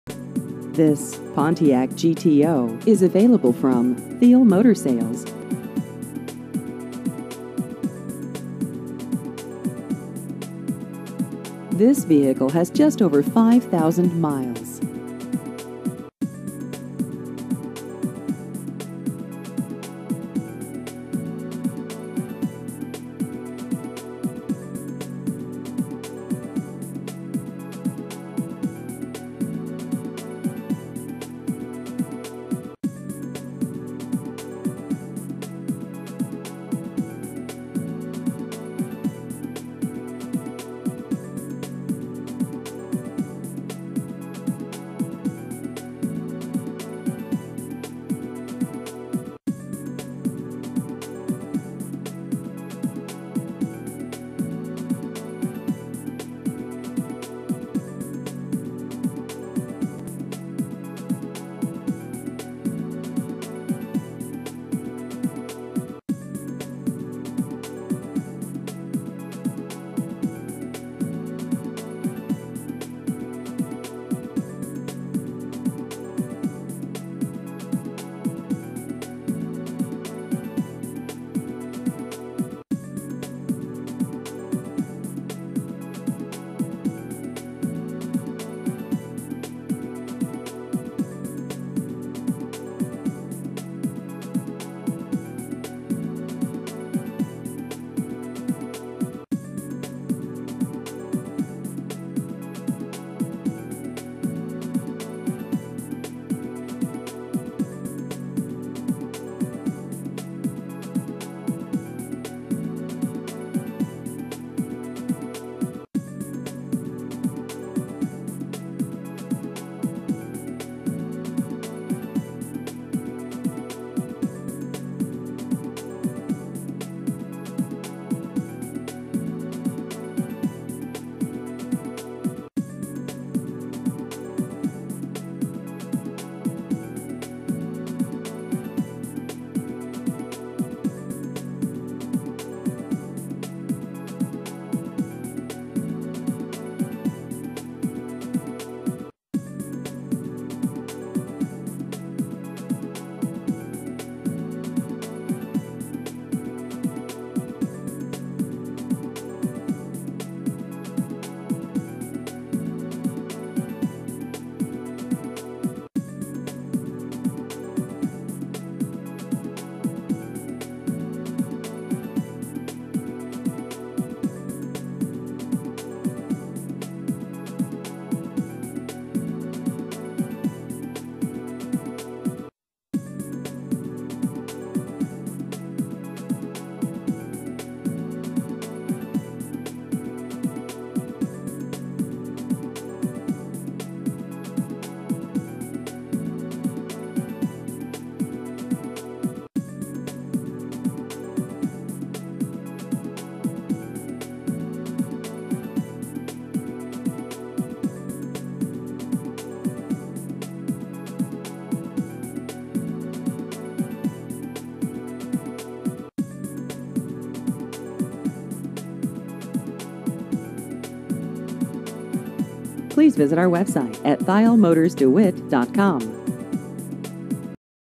1968 Pontiac GTO Used Cars De Witt IA
This 1968 Pontiac GTO is available from Thiel Motor Sales.